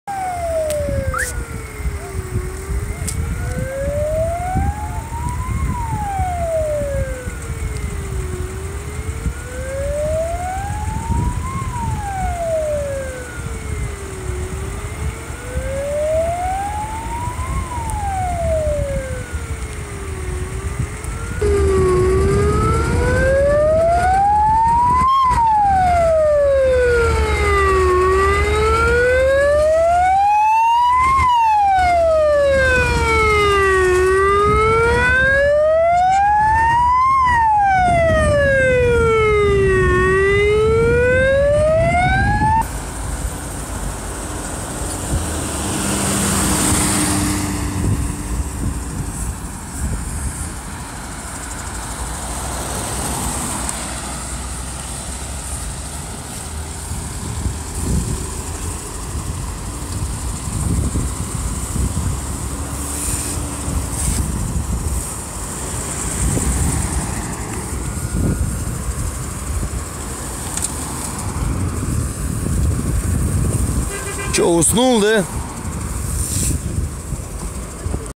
Камаз сгорел на трассе в Костанайской области
Камаз сгорел на трассе "Аулиеколь-Костанай". В Камазе было тюкованное сено. Произошло возгорание. Большегруз сгорел полностью.